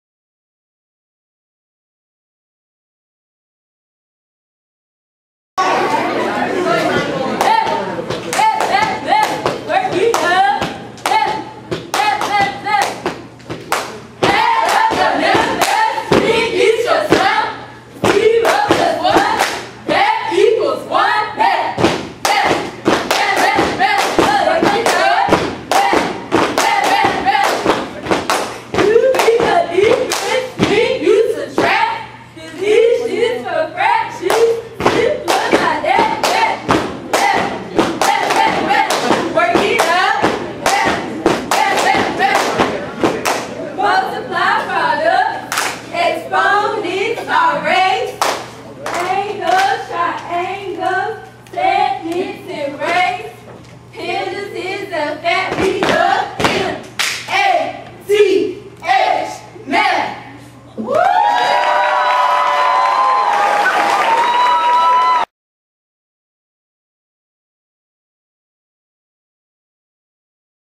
2013 Bertie STEM High School
Celebration of Women in Mathematics Math Cheer at Elizabeth City State University on October 15, 2013